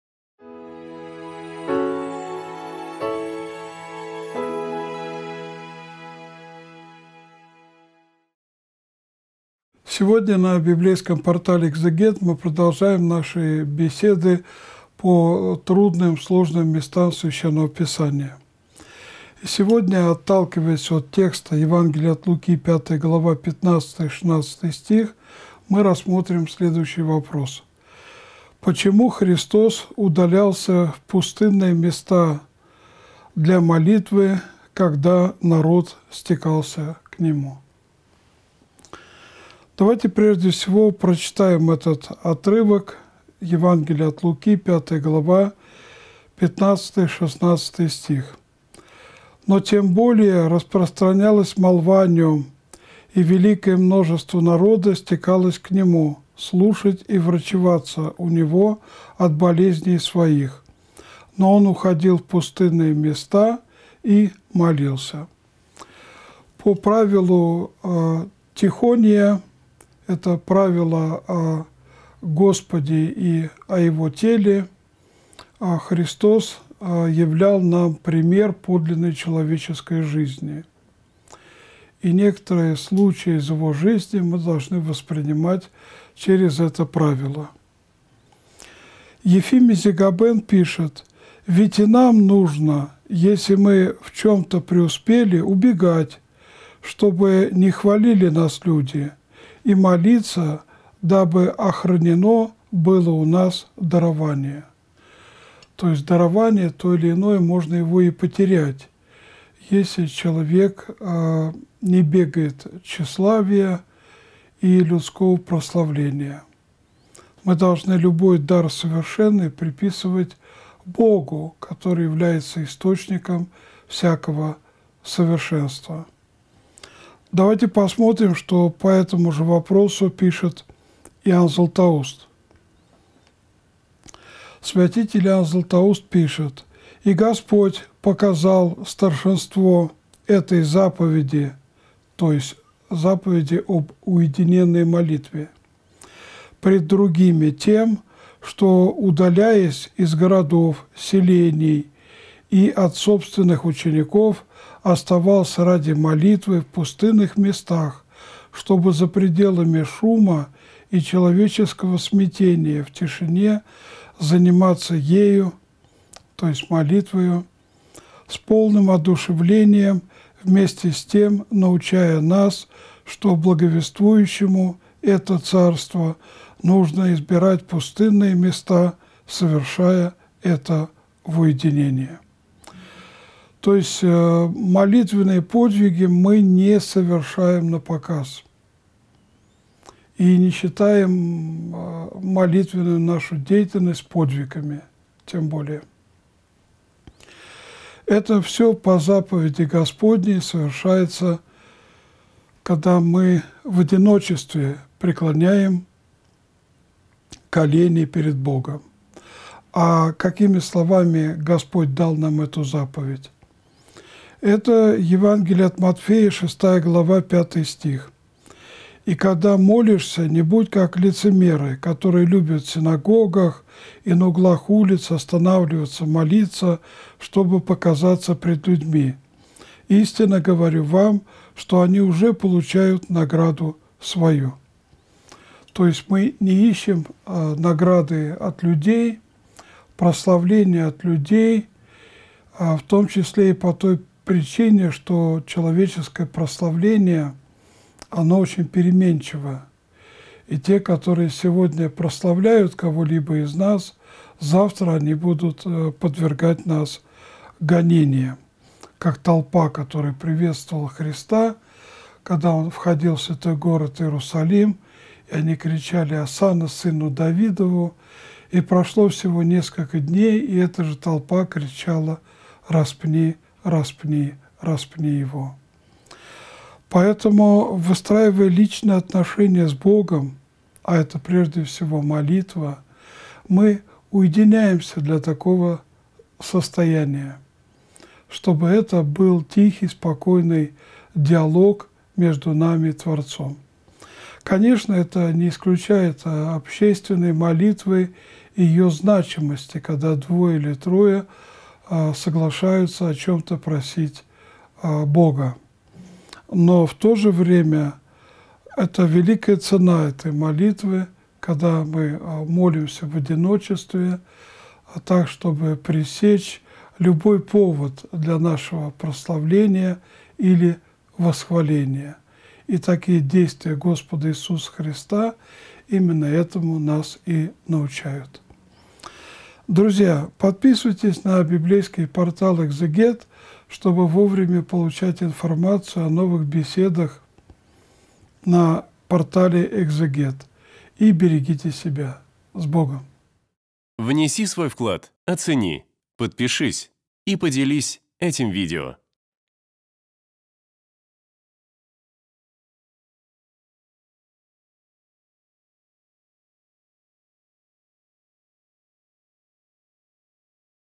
Почему Христос удалялся в пустынные места ? (Лк. 5:15-16) Протоиерей Олег Стеняев
В сегодняшнем видео с протоиереем Олегом Стеняевым разбираем очередное трудное место Священного Писания и отвечаем на вопрос: "Почему Христос удалялся в пустынные места?".

🔸 Смирение Христово.
🔸 Дарование Божие.
🔸 Комментарии Иоанна Златоуста.
🔸 Уединенная молитва.
🔸 Похвала окружающих.
🔸 Значимость общественной молитвы.
🔸 Христианская кротость.